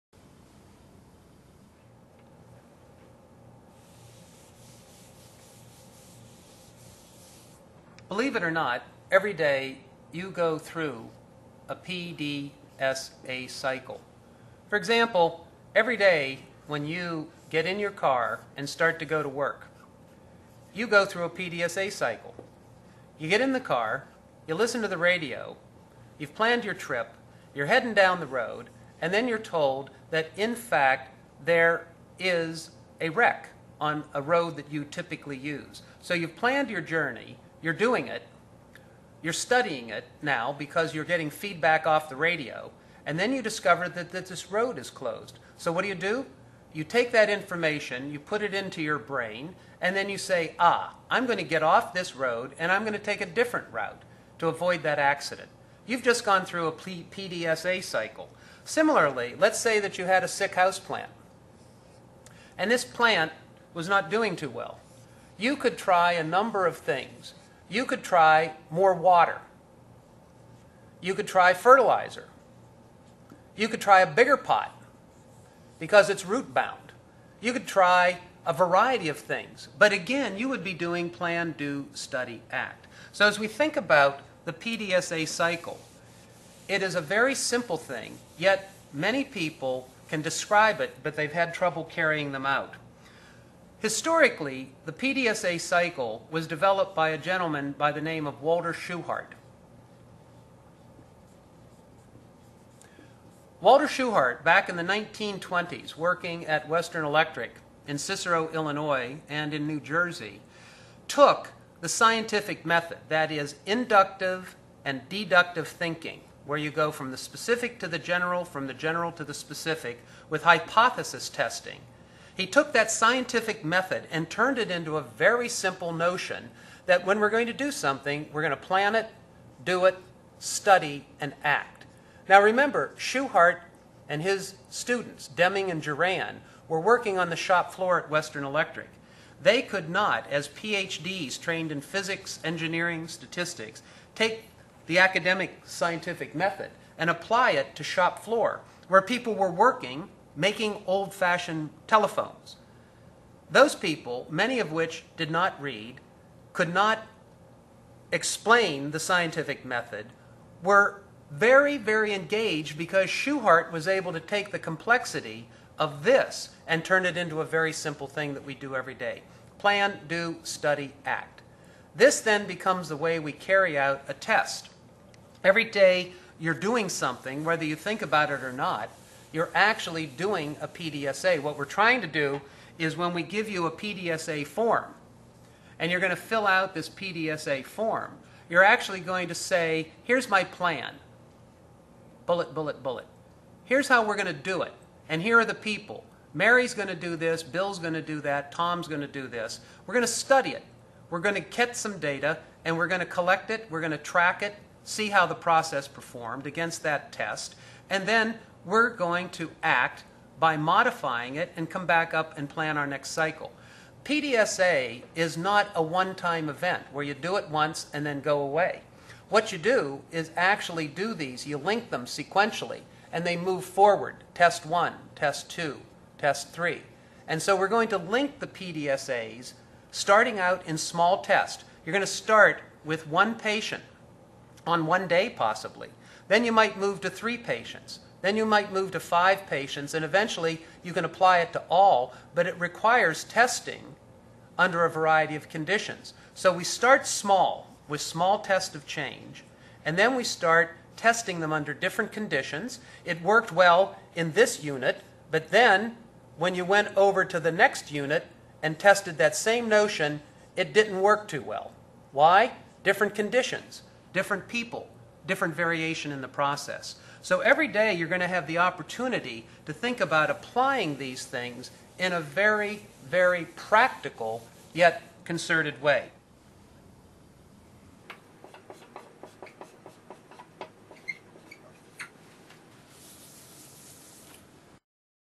Whiteboard: PDSA in Everyday Life
Robert Lloyd, PhD
From the IHI Open School's online course:
QI102 Lesson 5